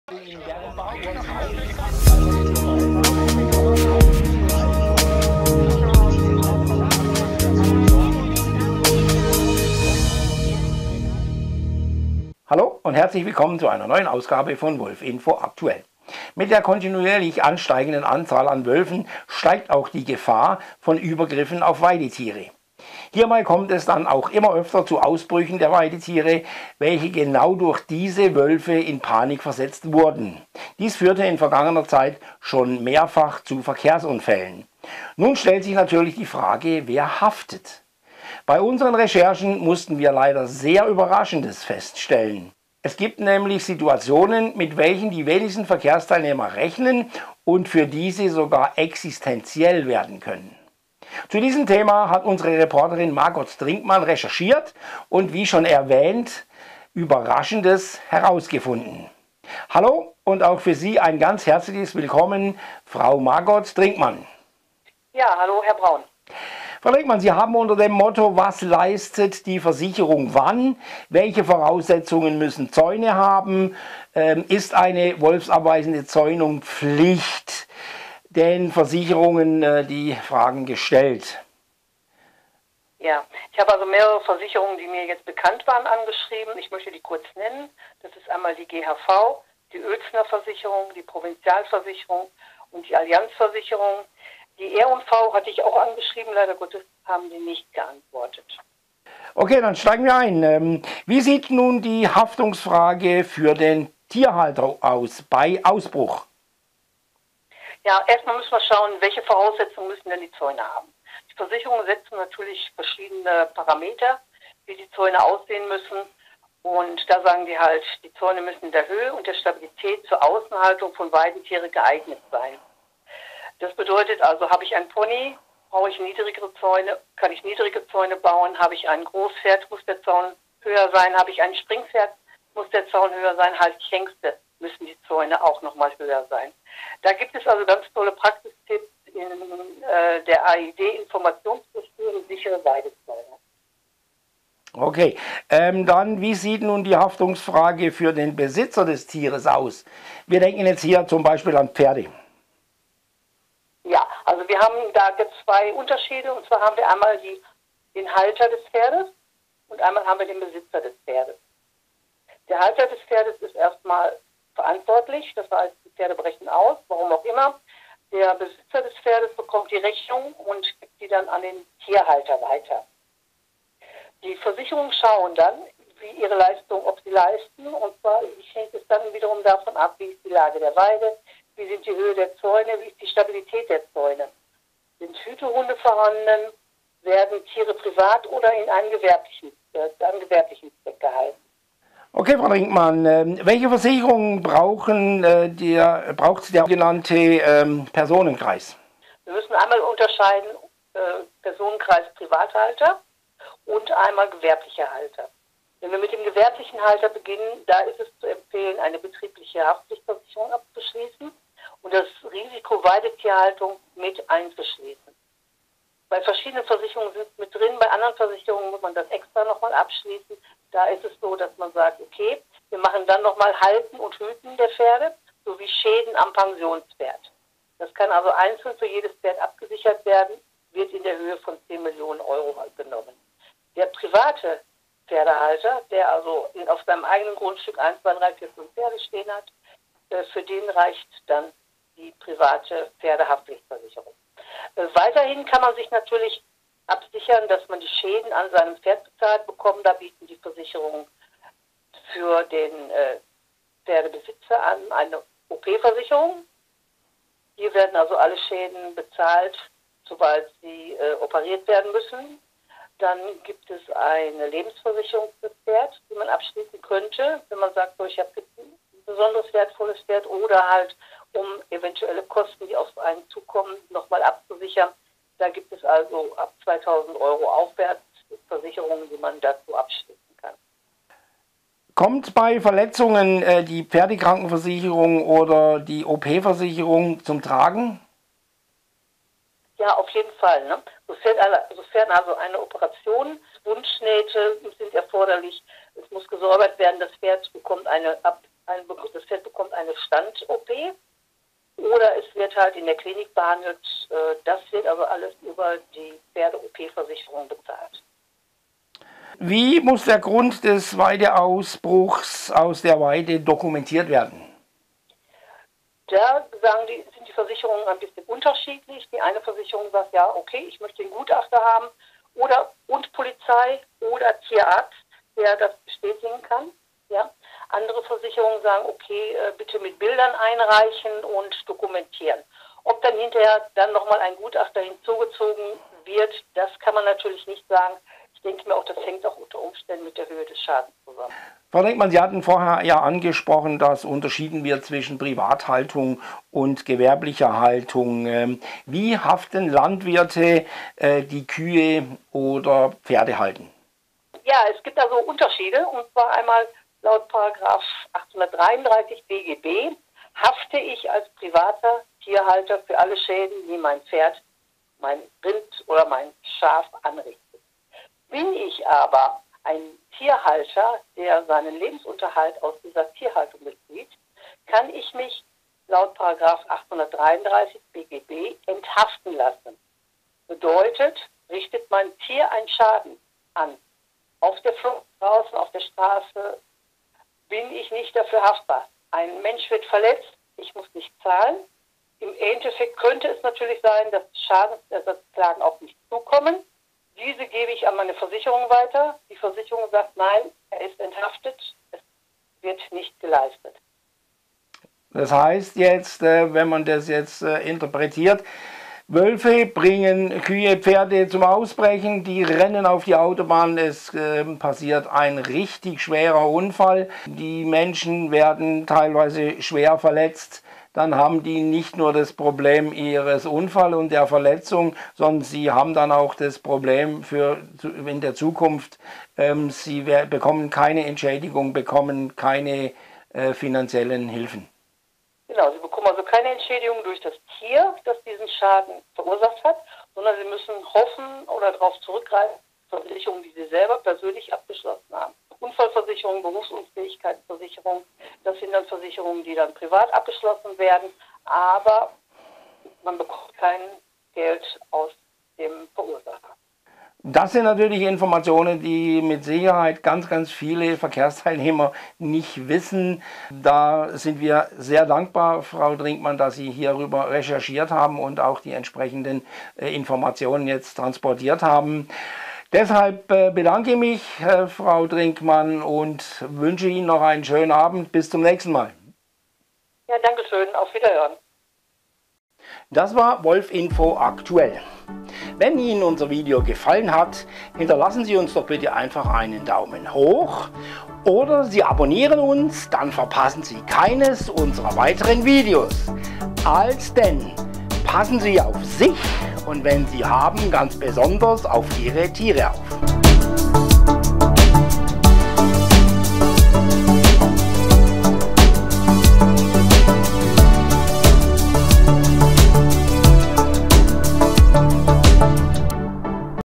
Wolfinfo Aktuell 86 ( Unfallopfer: durch ausgebrochen Pferde/Rinder keine Entschädigung.......)
Mit der rasant ansteigenden Wolfspopulation (Verdopplung alle 3 Jahre) steigt naturgemäß auch die Gefahr dass Wölfe, Weidetiere in Panik versetzten und diese aus ihren Einzäunungen ausbrechen. Wenn dann Straßen oder Schienen im Umkreis vorhanden sind,  kann und ist es auch schon mehrfach zu Unfällen gekommen. Sollte es deshalb zu einem sehr schweren Unfall mit verletzten Personen kommen, gibt es Situationen in welchen das Unfallopfer nicht nur den Schaden hat, sondern auch noch mit den daraus resultierenden Konsequenzen leben muß, sprich keine finanzielle Entschädigung bekommt.